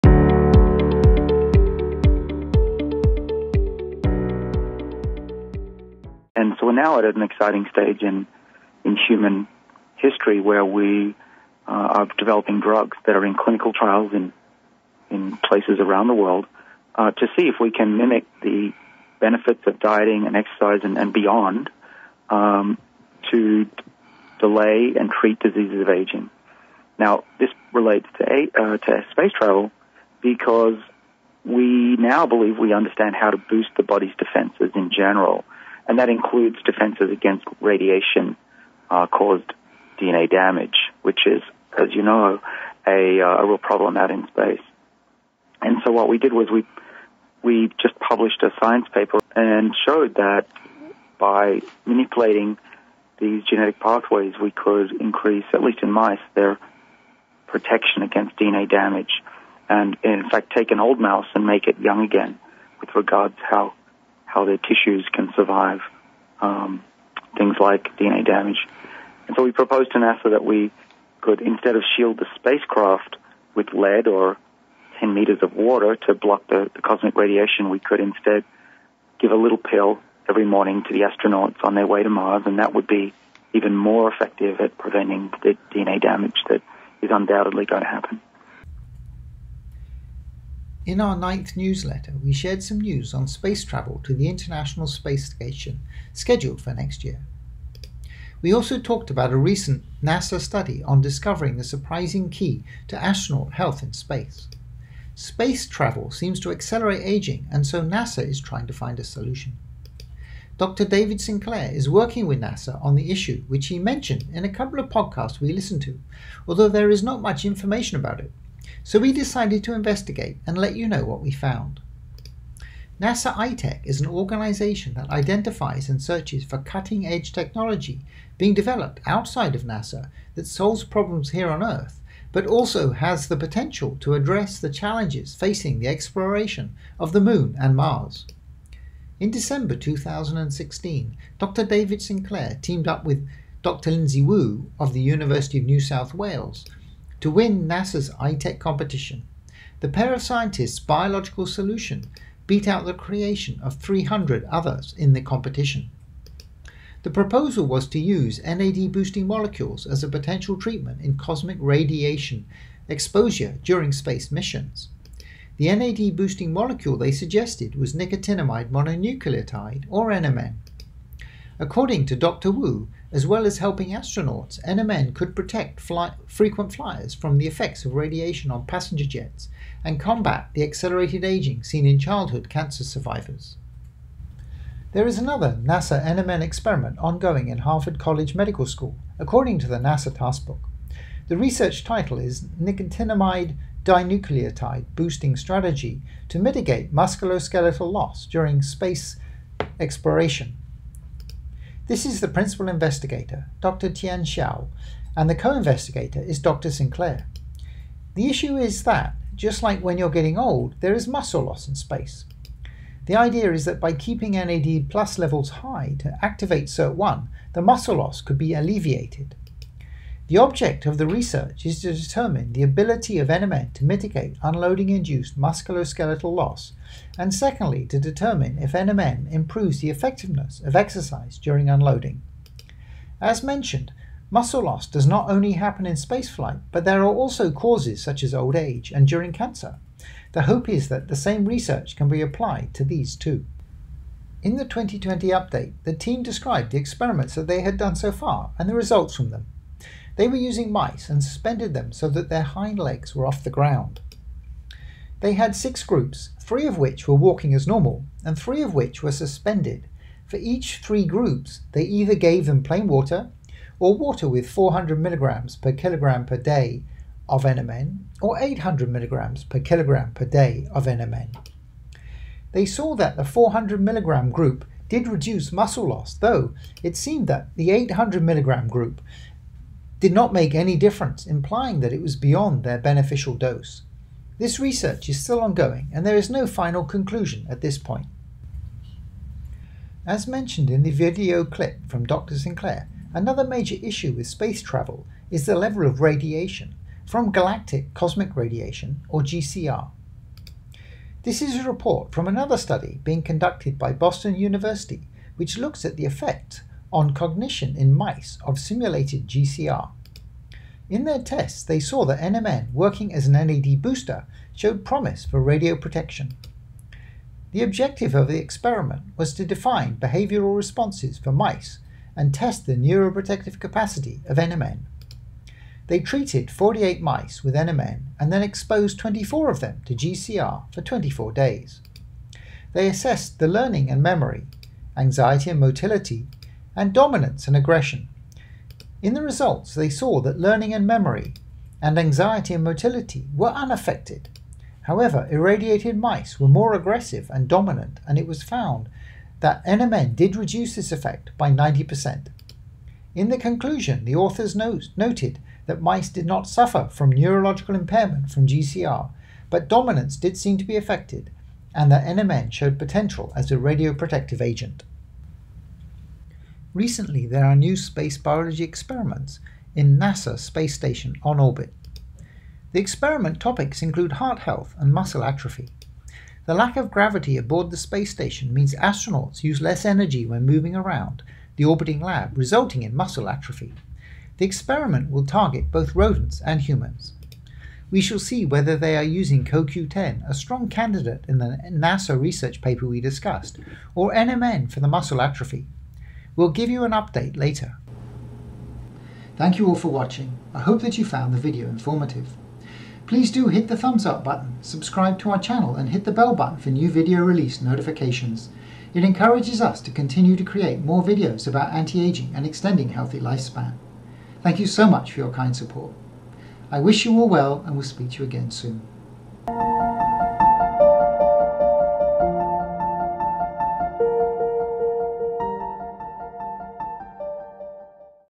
Dr. David Sinclair's NASA NMN Projects Review
Dr. David Sinclair proposes NMN to help NASA astronauts stay heatlhy in space.
Dr. Sinclair is working with NASA and proposing NMN to address two of the issues that astronauts face when traveling in space. One is the muscle atrophy that comes from not being in gravity and the other is the Galactic Cosmic Radiation which damages the DNA. NMN by supporting DNA repair and inhibiting protein degratdation.

Project Title:  Nicotinamide Dinucleotide (NAD)-Boosting Strategy to Mitigate Musculoskeletal Loss During Space Exploration

Investigation of neuroprotection by NAD+ in response to deep space radiation

Comprehensive Multi-omics Analysis Reveals Mitochondrial Stress as a Central Biological Hub for Spaceflight Impact